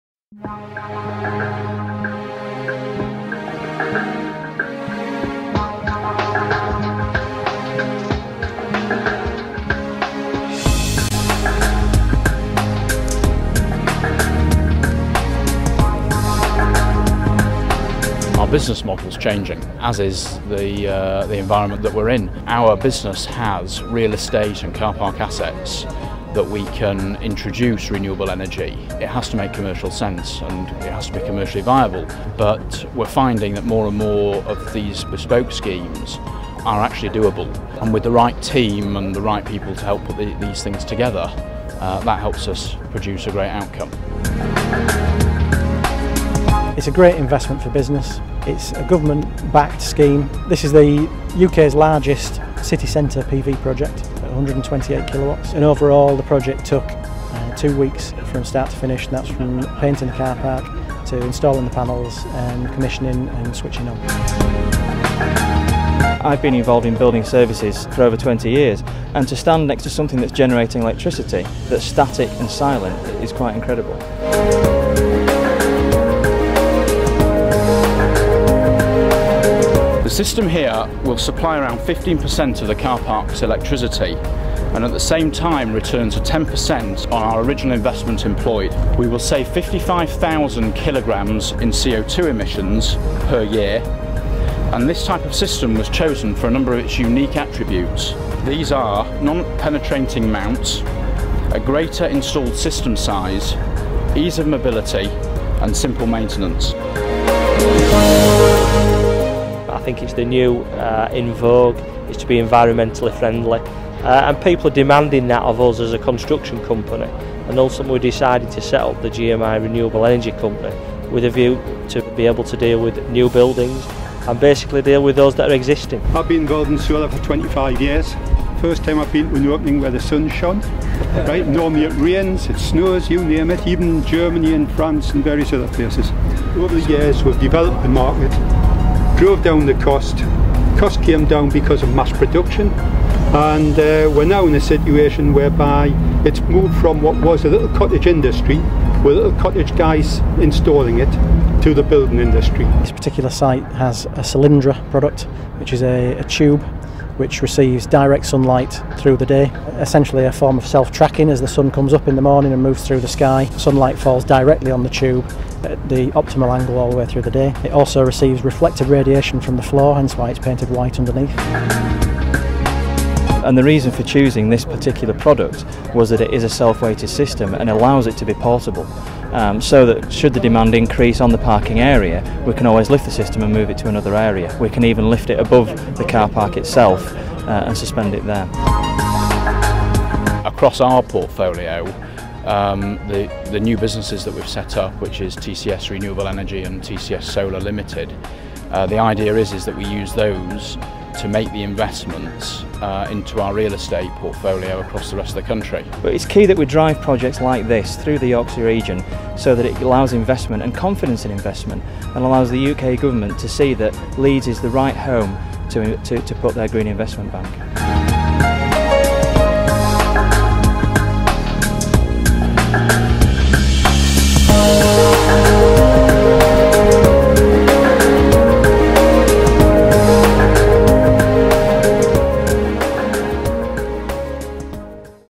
Solar Panels
A NEW solar panel system on a car park roof in Leeds is the largest in any UK city.
Town Centre Securities launched the system on the seventh floor of its Clarence Dock multi-storey, next to the Royal Armouries.

The firm has worked with Rex Procter & Partners and GMI Renewable Energy Group to construct the "unique" system, which utilises an array of tubular panels instead of flat ones. This makes them more efficient.

The photovoltaic system can even withstand 130mph winds and a battering by 25mm hailstones.

Paul Osborne, operations and maintenance manager at GMI, told the YEP: "This is the largest city centre solar array for this country."